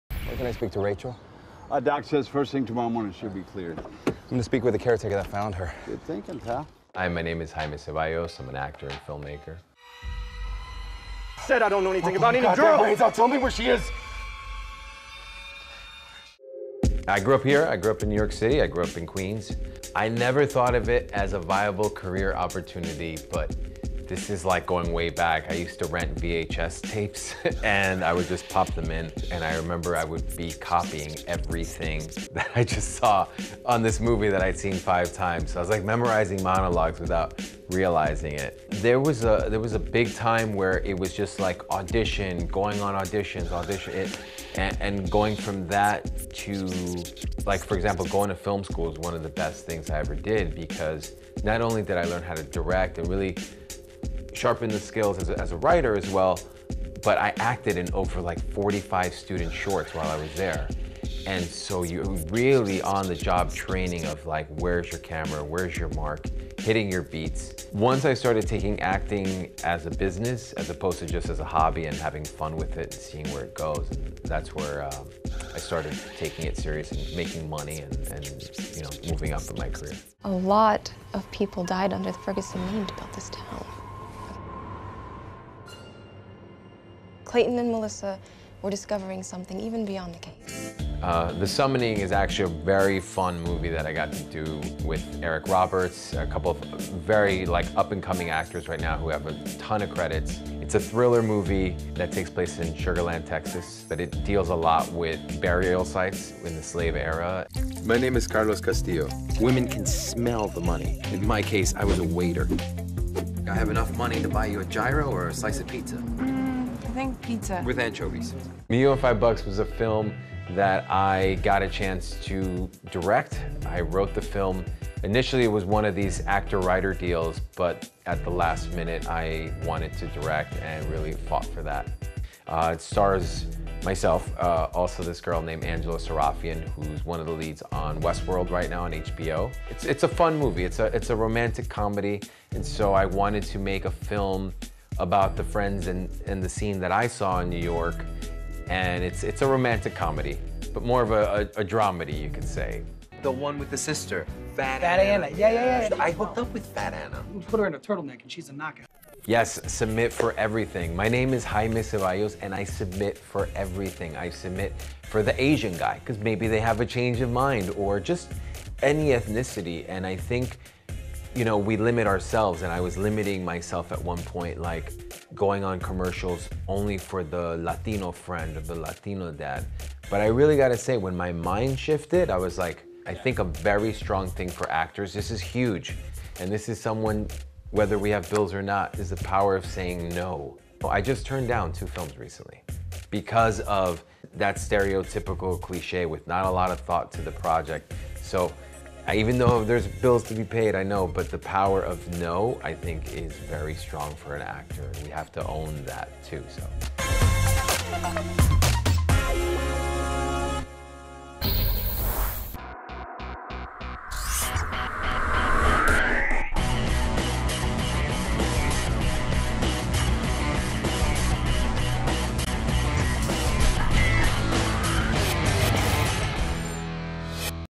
Jaime Zevallos: behind the scenes of Cloak & Dagger | LatiNation
We have actor Jaime Zevallos, He’s an all-around entertainment hustler, a writer, producer, director, and actor. Jaime has been pounding the payment for years as a seasoned indy actor and director, but his work really paid off when he joined the Marvel universe as one of the lead characters of Marvel’s Cloak and Dagger, a TV adaptation of the popular comic book airing on the Freeform Network. And we get to find our more, right here on LatiNation.